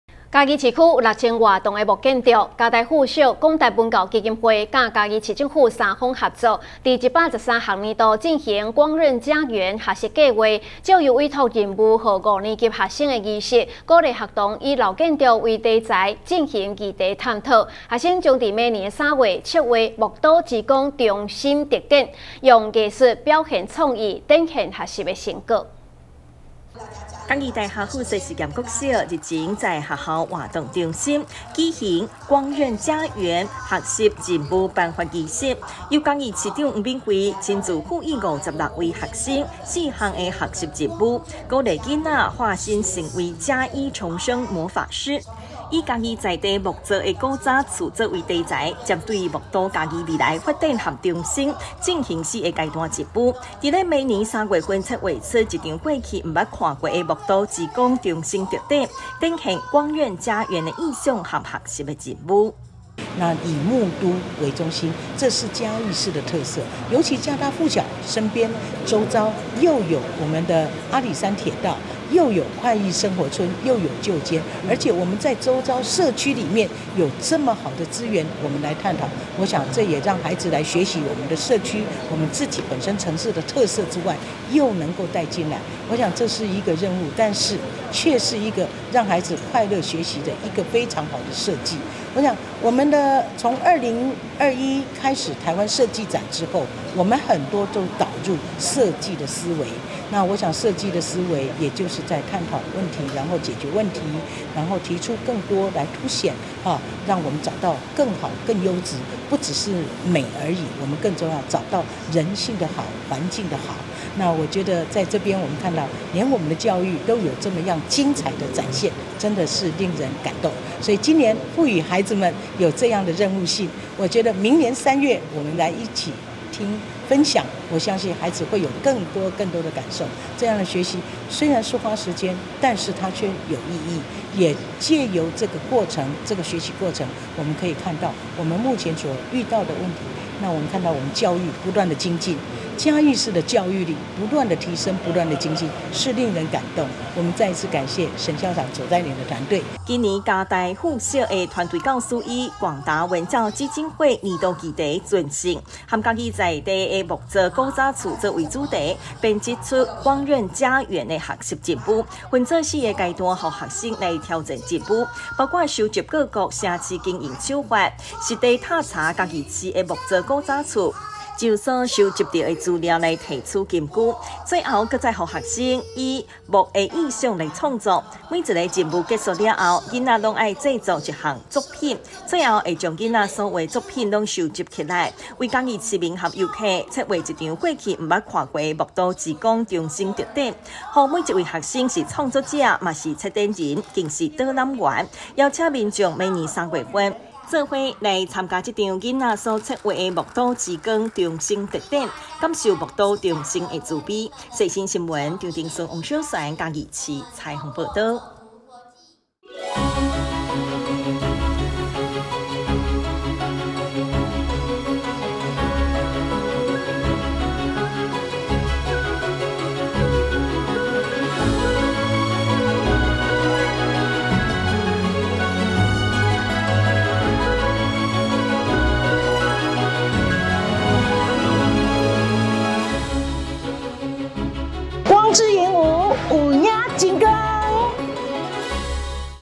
世新新聞 光韌嘉園！學童化身”重生魔法師” 老木屋注入新生命
113年9月19日 (四) 世新新聞

光韌嘉園！學童化身”重生魔法師” 老木屋注入新生命

嘉義市區有6000多棟木屋，有「木都」美名，嘉大附小、廣達文教基金會與嘉義市政府三方合作，在113學年度進行「『光』韌家園學習計畫」，藉由委託任務給五年級學生的儀式，鼓勵學童以老木屋為題材進行議題探究。學生將在明年3月策劃「木都之光」重生特展，用藝術表現創意，展現學習成果。